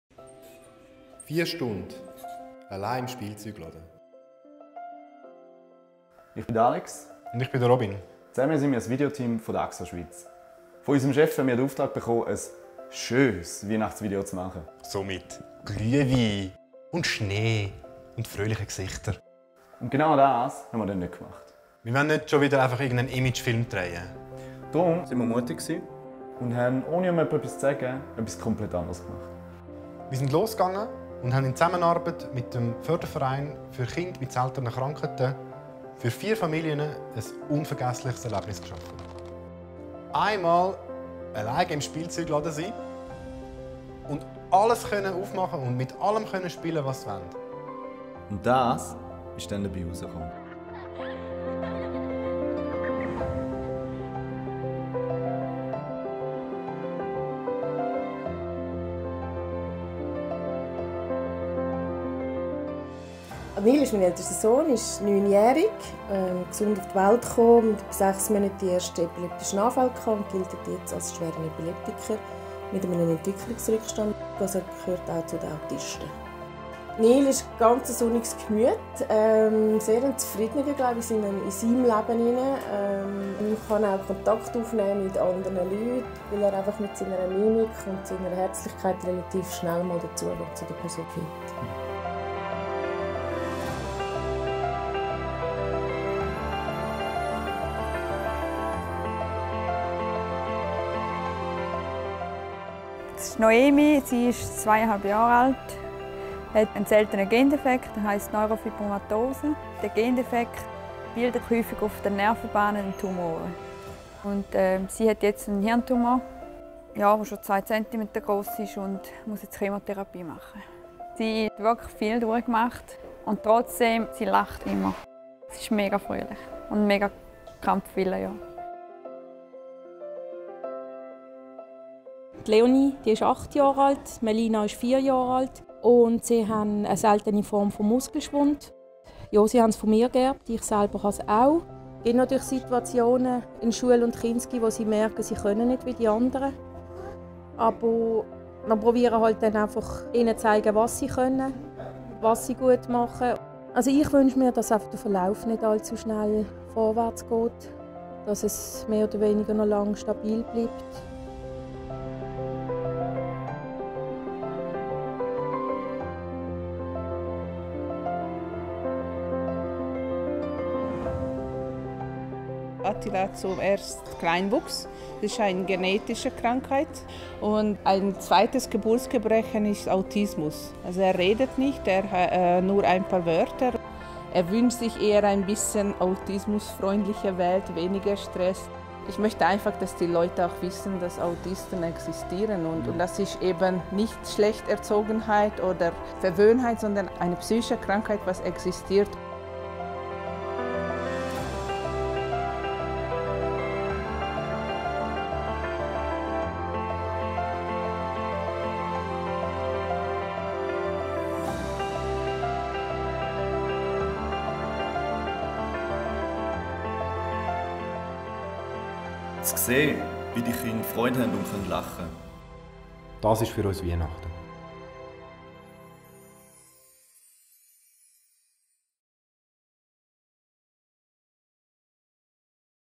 Seltene Krankheiten – Weihnachtsvideo
Ein verschneiter Weihnachtsmarkt, Glühwein und lachende Gesichter – so hätte das Weihnachtsvideo der AXA Schweiz aussehen sollen. Hätte – denn das Videoteam rund um Robin Flückiger und Alexander Wegmann hatte eine andere Idee, zeigte Mut und setzte diese statt der Vorgaben ihres Chefs um. Statt eines Hochglanzvideos wollten Robin und Alexander viel lieber Kindern, die an einer seltenen Krankheit leiden, ein besonderes Weihnachtserlebnis schenken. In Zusammenarbeit mit dem Förderverein für Kinder mit seltenen Krankheiten wurde dieser Wunsch erfüllt. Und so durften fünf betroffene Kinder und ihre Familien einen Abend ganz allein im Spielzeugparadies geniessen, mit Eisenbahnen und Puppen spielen und einfach Kind sein.